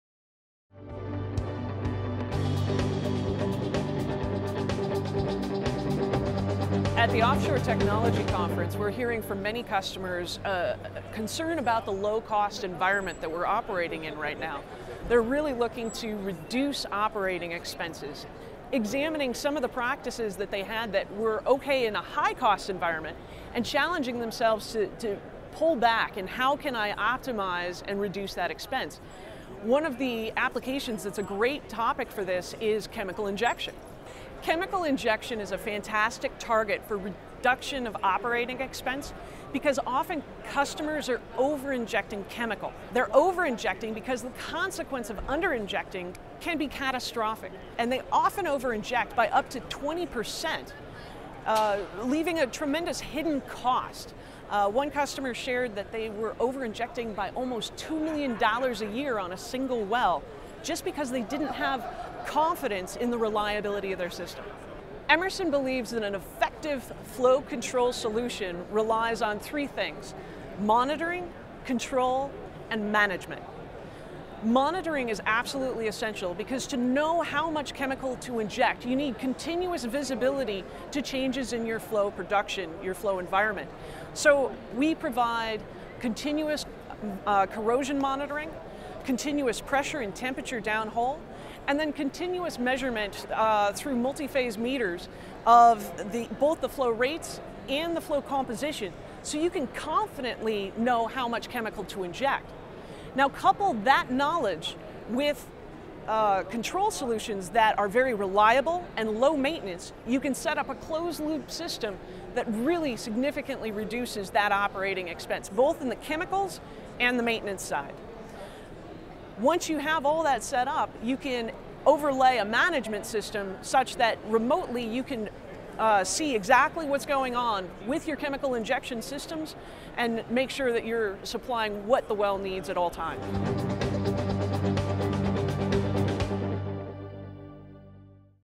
Optimizing Well Chemical Injection through Flow Assurance
Emerson's Laura Schafer describes how oil & gas producers are looking to reduce production costs in a low price environment by optimizing chemical injection costs.